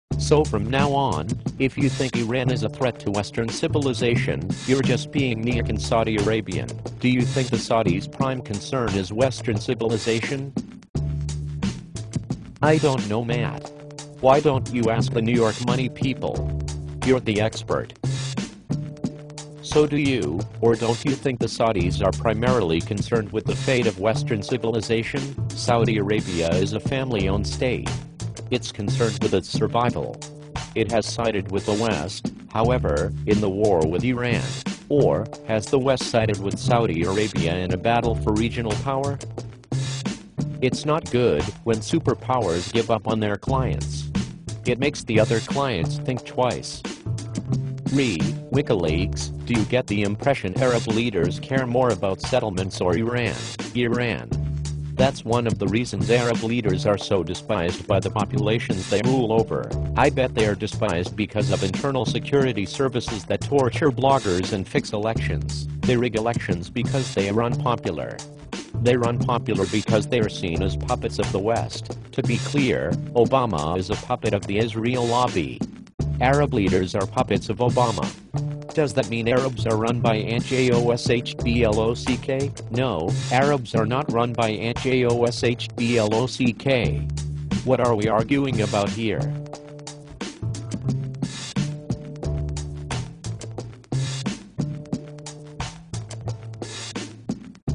Eli Lake and Matt Yglesias as two CGI bears, w/hip hop soundtrack
Lake and Yglesias debate Wikileaks, Iran and the influence of the Israel lobby